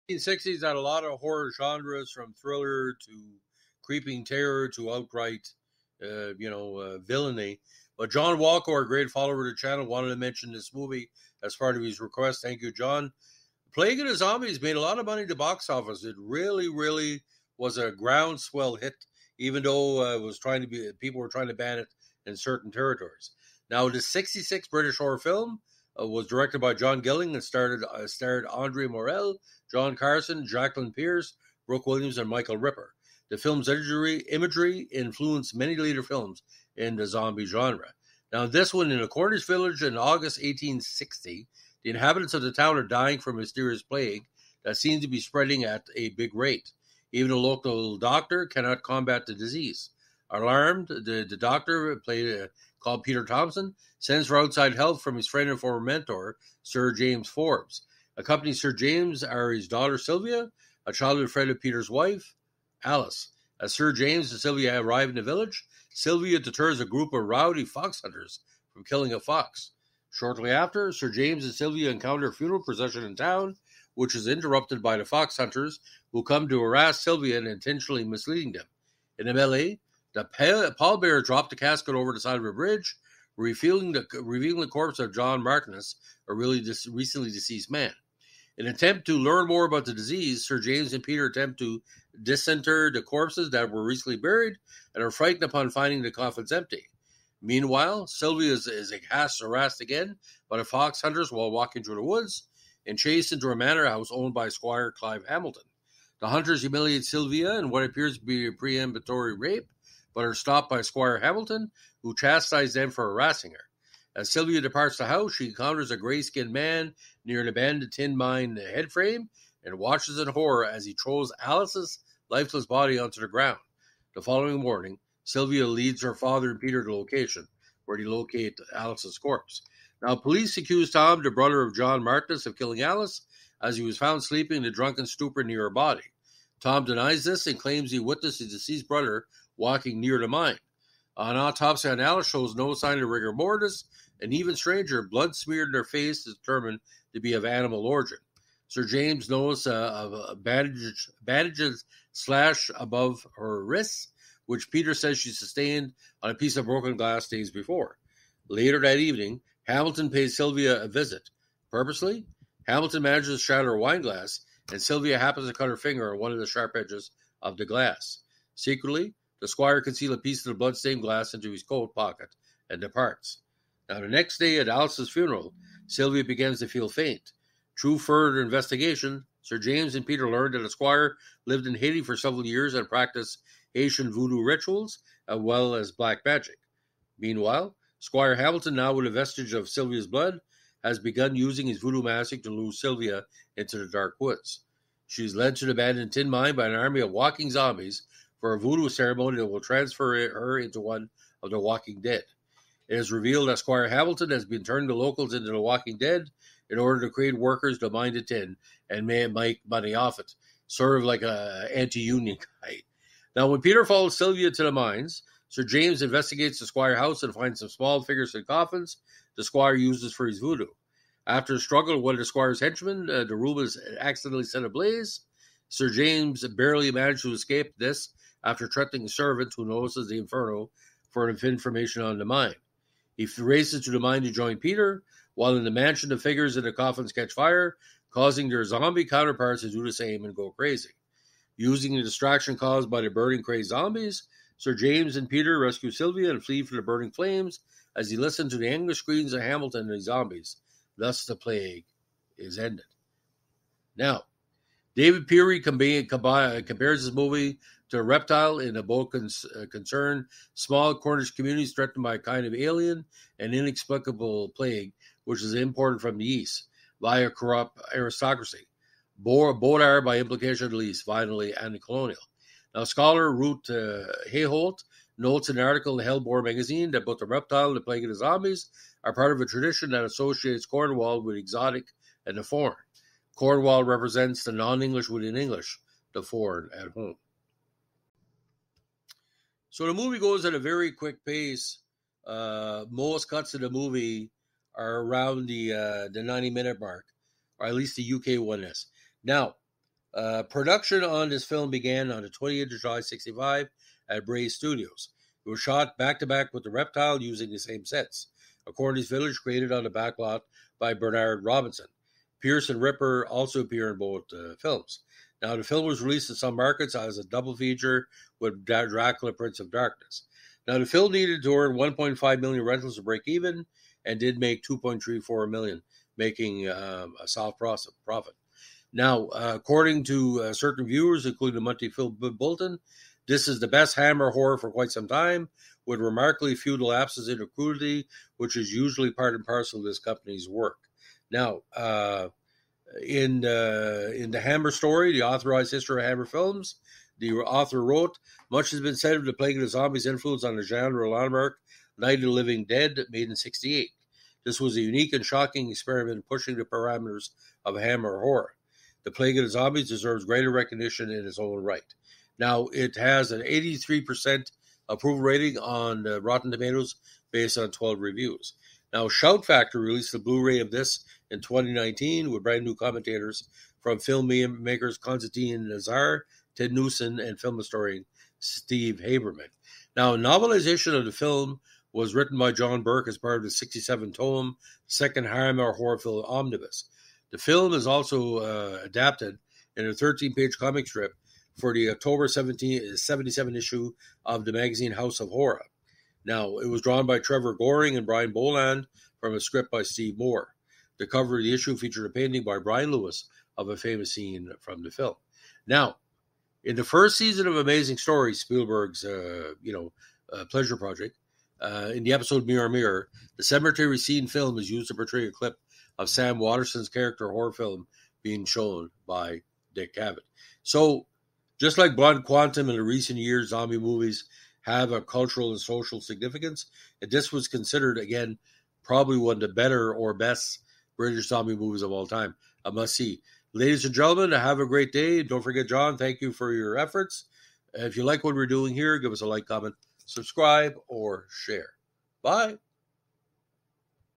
The Plague Of The Zombies (1966) Was A Groundbreaking Film In The British Horror Industry
The Hammer flick inspired many walking dead movies and TV shows that followed as it brought new 'undead' life to the genre